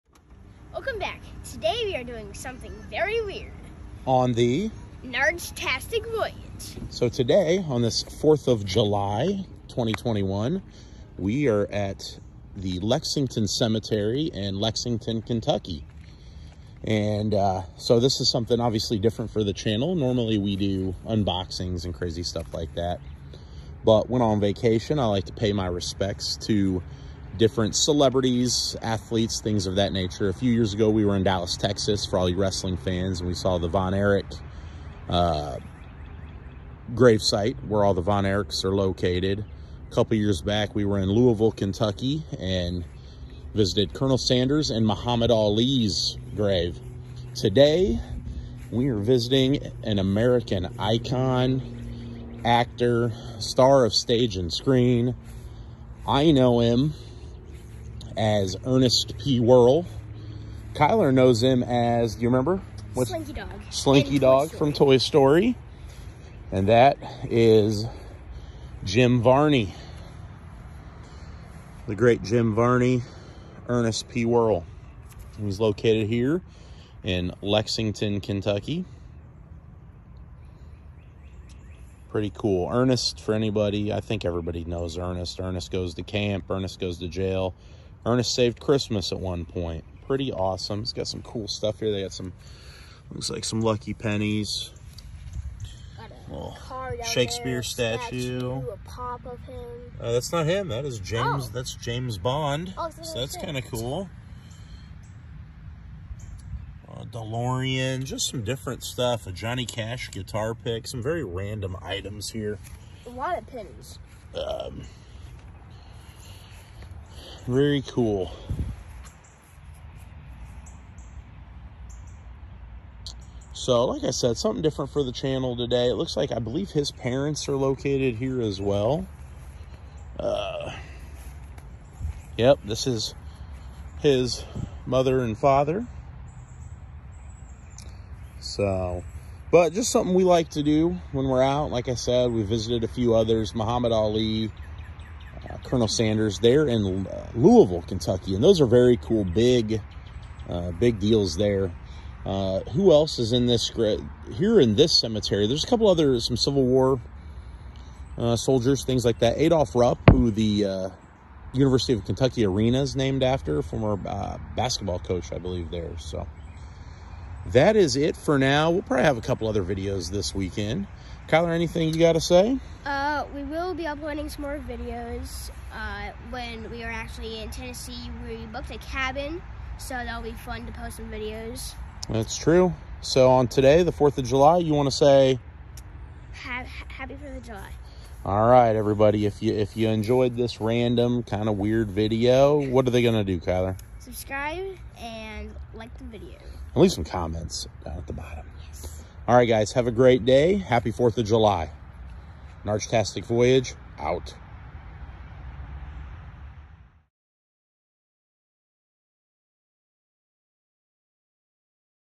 The Grave Site of Jim Varney Jr aka Ernest P Worrell
A quick visit to pay respects to actor Jim Varney at his final resting place, the Lexington Cemetery in Lexington, KY

Mr Varney was famous for his character Ernest P Worrell and the voice of Slinky Dog in Toy Story